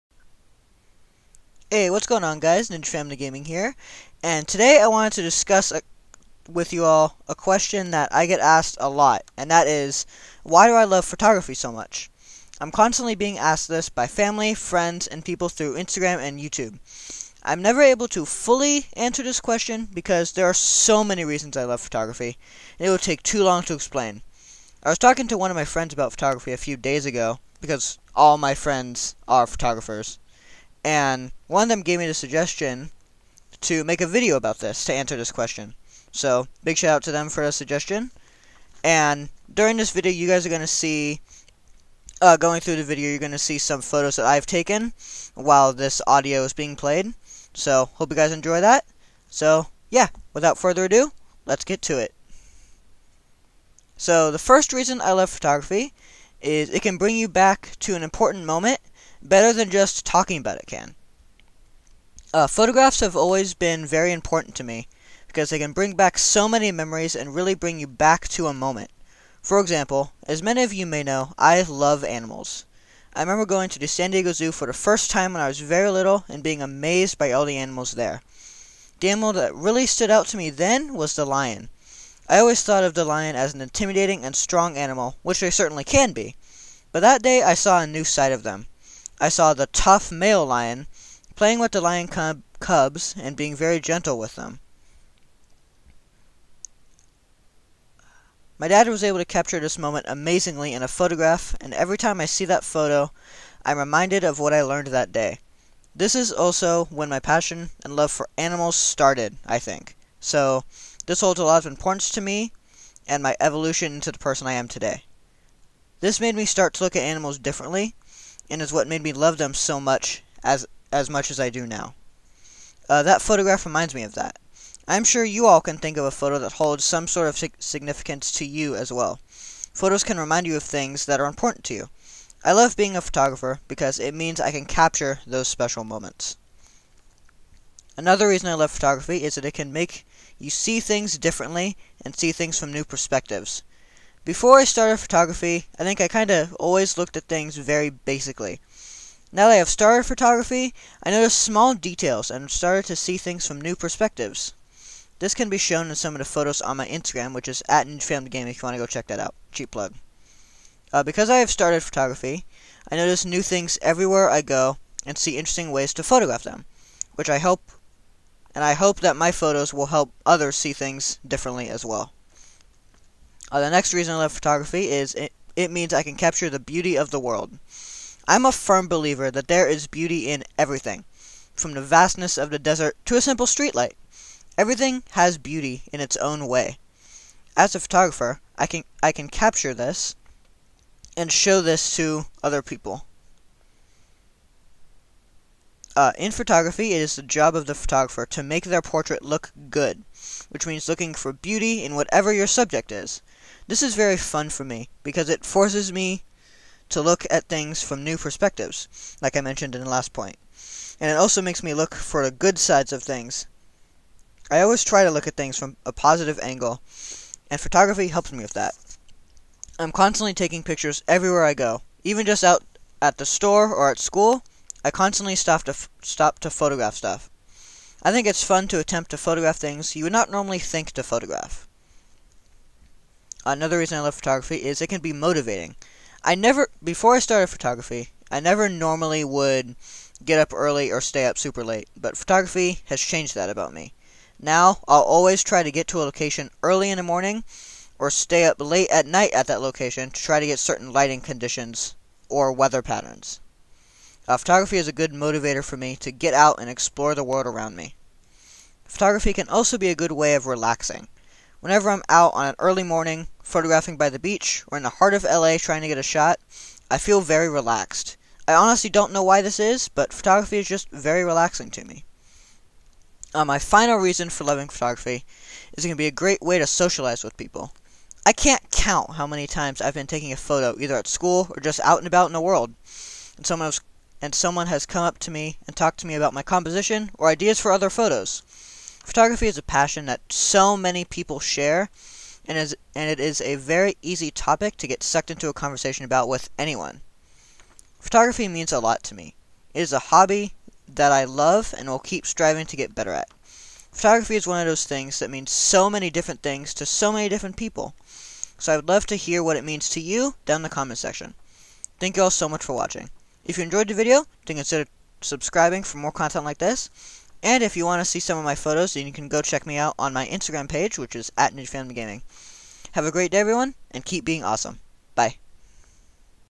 Reason why I love photography.
In this video I am going to be telling you all why I love photography. This is a question I get asked a lot so I hope this will answer that question. Hope you enjoy the video.